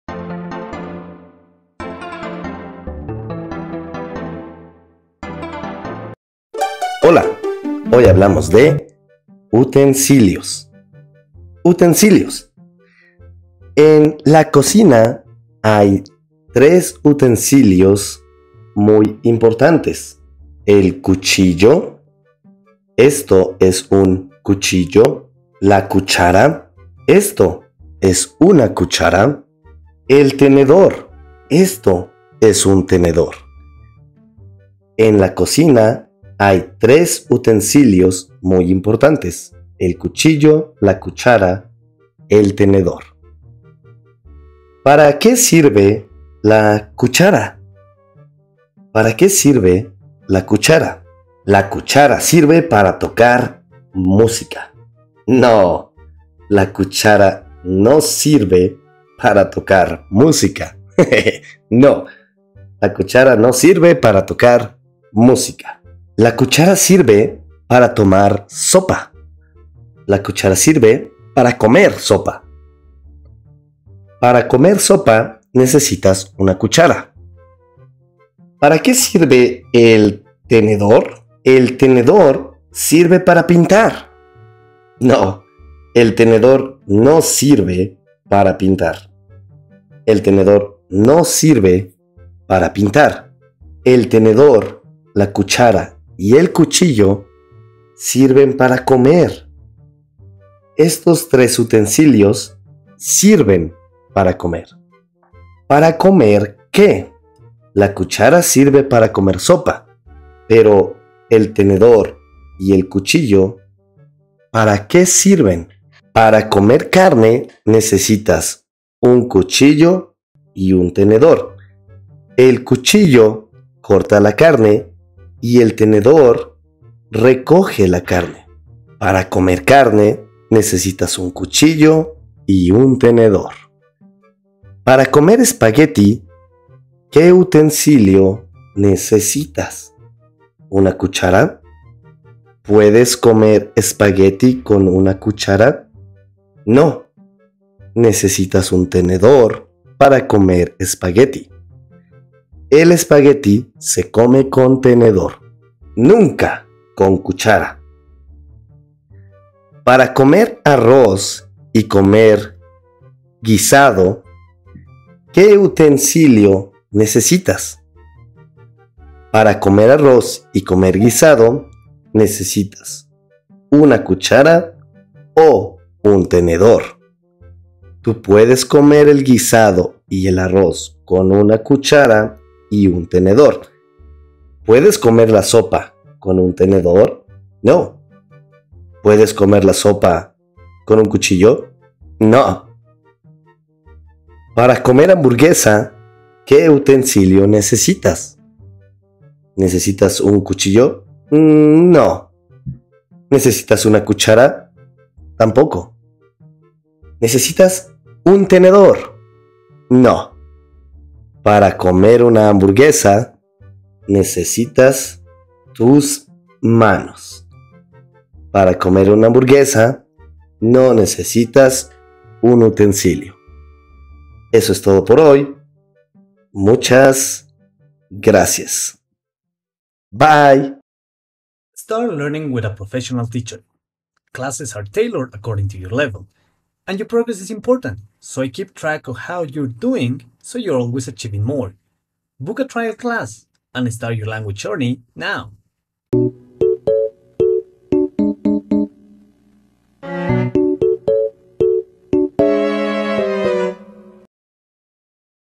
Utensilios - SUPER EASY Comprehensible Input
SUPER EASY Comprehensible in Spanish is a series of videos where the language is manageable and super easy to understand with images or visual representations.

Complete Spanish Grammar Third Edition
Complete Spanish Grammar Fourth Edition